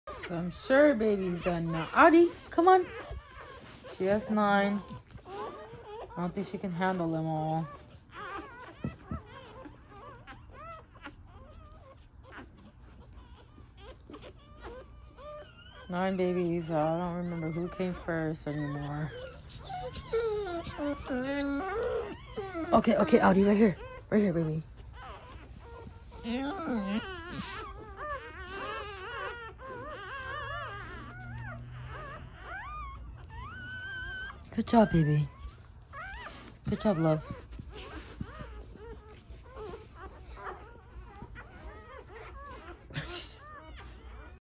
Bluenose pitbull gives birth to 13 pitbullys!!! Pt. 5
Audi and Her Baker’s Dozen